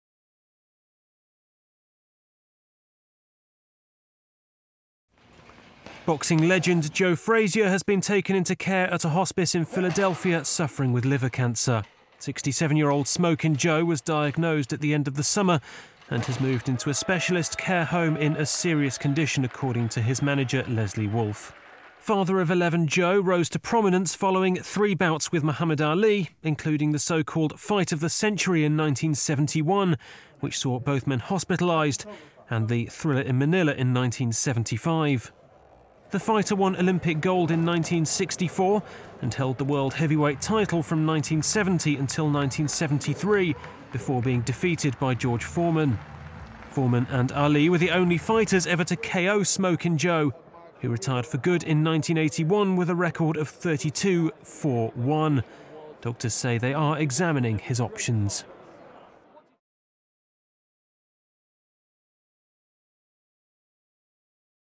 SMOKIN' JOE: Boxer Joe Frazier in hospice with liver cancer
Boxing legend Joe Frazier, who famously fought Muhammad Ali in the Thrilla in Manila, has been moved into a specialist care home in Philadelphia suffering from liver cancer. Report by Sam Datta-Paulin.